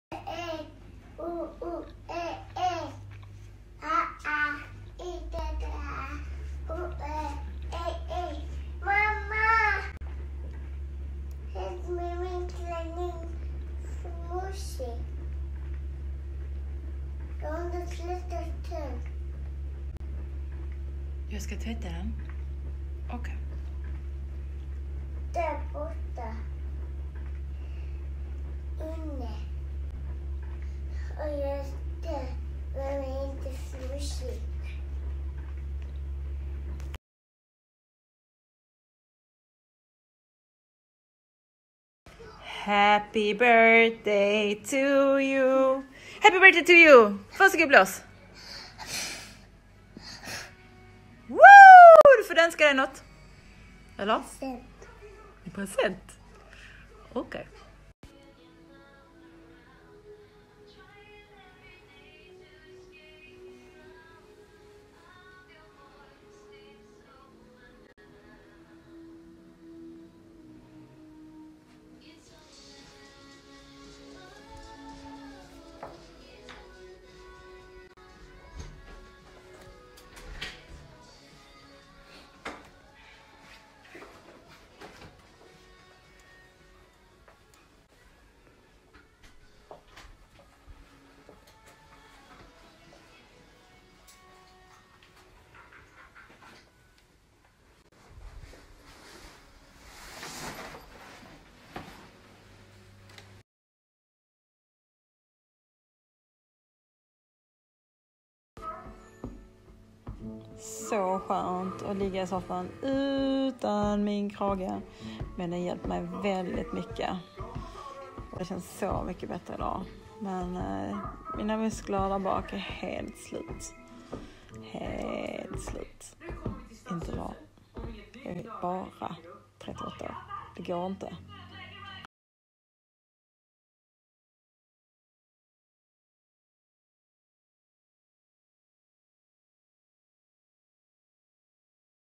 My day 2020.01.22
It’s just a normal day except that I have problems with my neck 😖

It’s the 22 of January and it’s Kataras date of birth, we are celebrating with a 🎂 🥳😂

I don’t get anything done but I’m grateful for all help I get 🙈
I love when it’s clean, my soul feels free and there is a bigger smile on my face 😁🤣